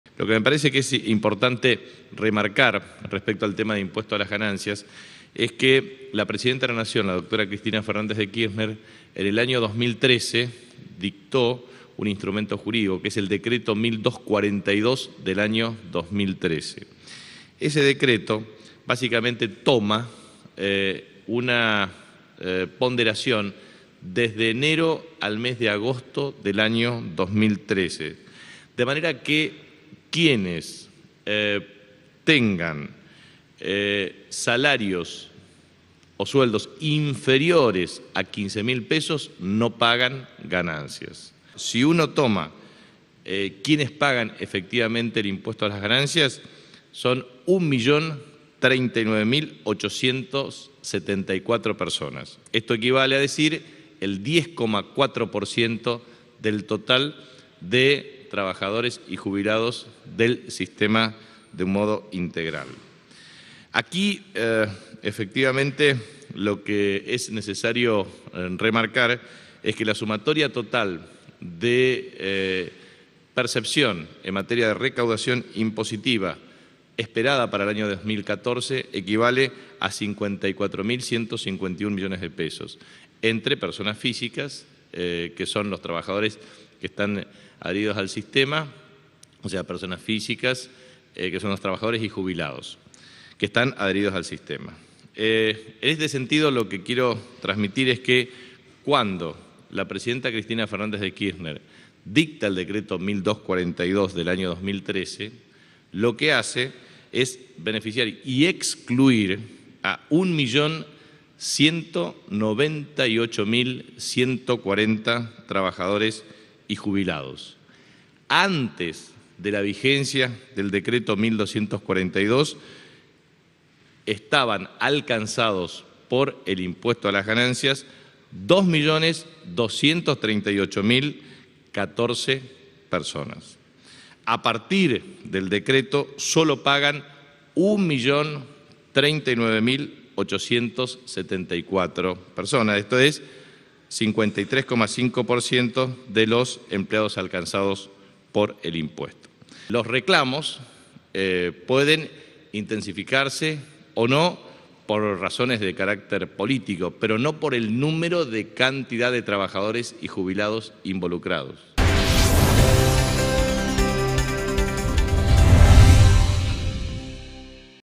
Capitanich: "el reclamo por el impuesto a las ganancias es político"
El jefe de Gabinete, Jorge Capitanich, consideró hoy que el reclamo gremial referido al aumento del mínimo no imponible del impuesto a las Ganancias es "político", ya que "los trabajadores que no estaban alcanzados" por este gravamen "siguen sin ser alcanzados, independientemente del incremento de su salario" por las paritarias.
Capitanich hizo estas declaraciones en su habitual conferencia de prensa matutina en Casa Gobierno, al dar cuenta de la reunión que ayer mantuvo con dirigentes la CGT que conduce Antonio Caló.
Según señaló el funcionario, "el impuesto a las Ganancias afecta al 10,9 por ciento de los trabajadores", que, añadió, son aquellos cuyos ingresos son superiores a 15.000 pesos.